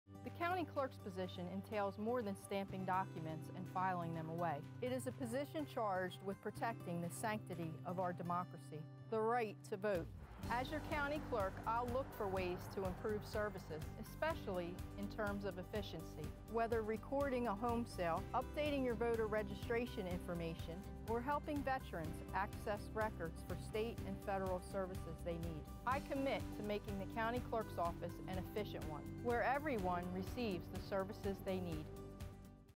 Vote Jiampetti for County Clerk
The County Clerk is responsible for protecting our right to vote among other services. I'll increase efficiency, making certain our veterans have the documents they need, property records are recorded properly, and voting runs smoothly. Vote Jiampetti for Clerk on November 2nd.